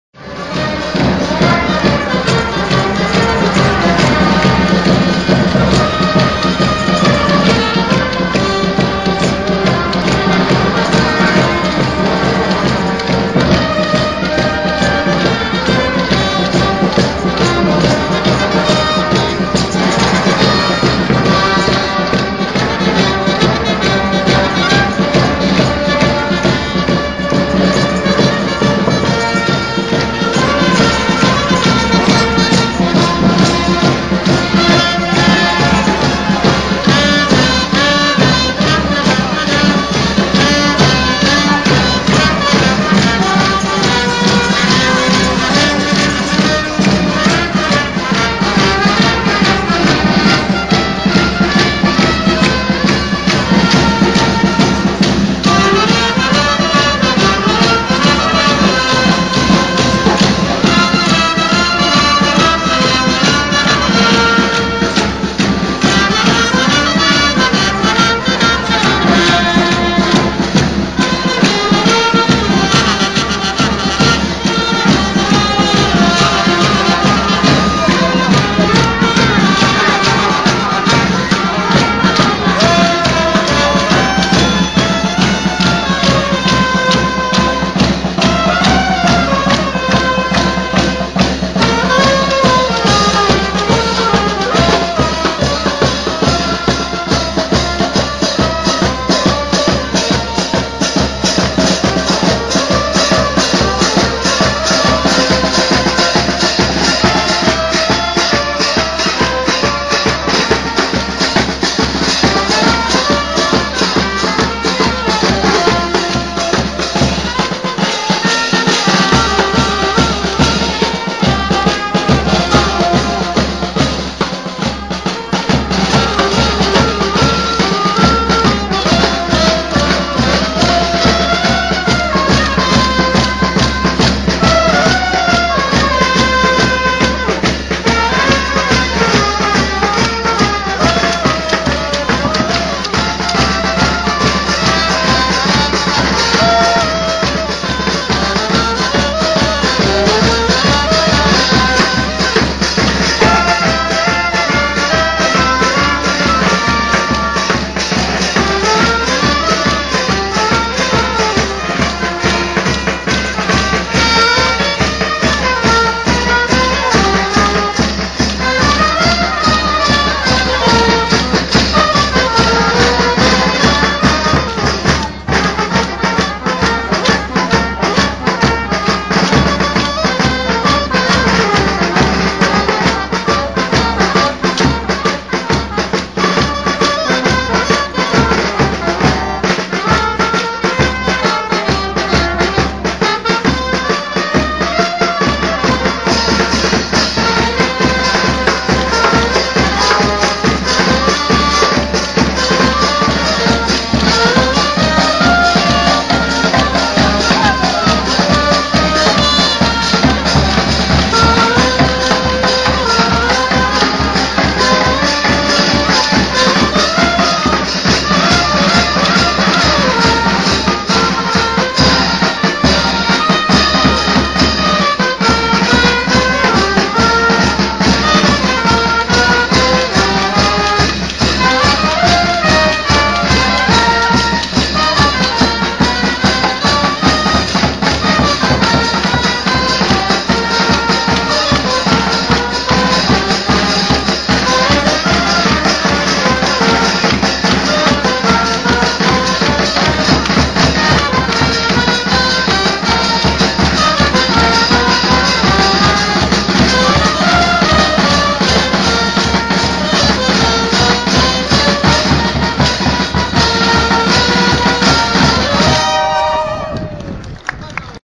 1 gennaio 2008 - Pro Loco Grumo Nevano - Napoli
Ecco come si è festeggiato il primo gennaio a Grumo Nevano Napoli